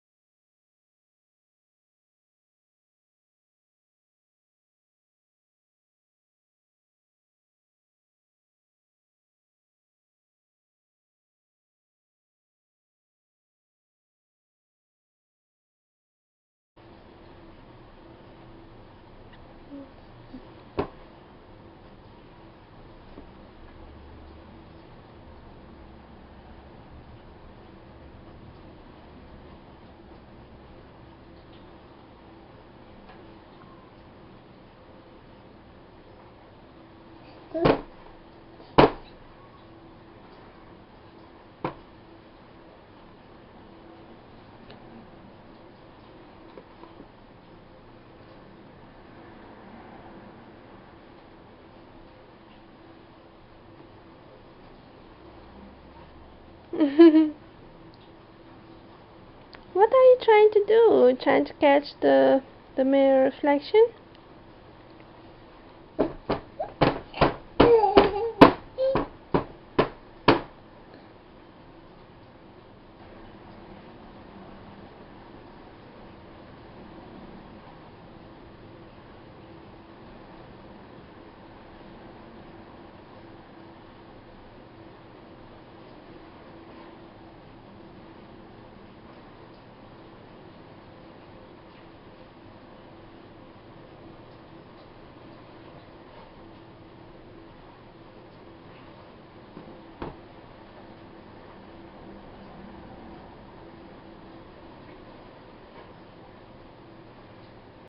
Funny Baby Trying to Catch Mirror Reflection!
I could not believe my eyes ! When I saw him trying to catch the mirror light reflection! Smart 7 month old baby ! Funny Baby Chases Mirror Reflection!

I WAS surprised and confused, this is Amazing! My baby chases light reflection from Mirror, he is so cute! And Funny, I noticed every time he saw something he want to hold this, but he couldn't catch the light here he seems so curious and confused, he is trying to understand what is going on, but he couldn't understand it! LOL